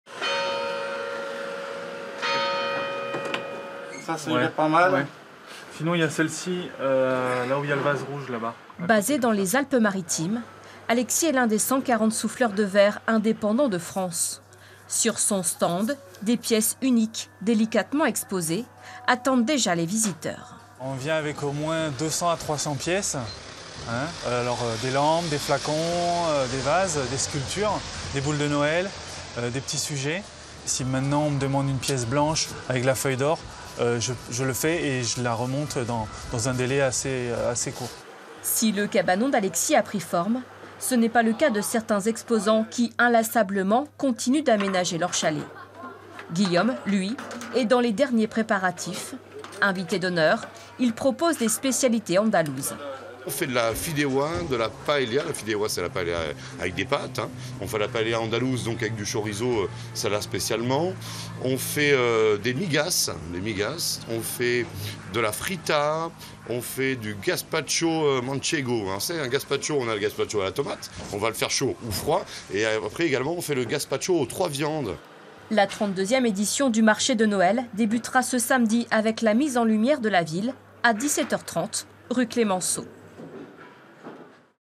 Marché de Noël de Montbéliard : zoom sur les derniers préparatifs
Intervenants : Alexis Mathelin - Souffleur de verre, Guillaume Foata - Cuisinier de spécialités andalouses.

Reportage : HIRSON Vanessa, POIROT Rémy et MAUJARD Jean-Pascal.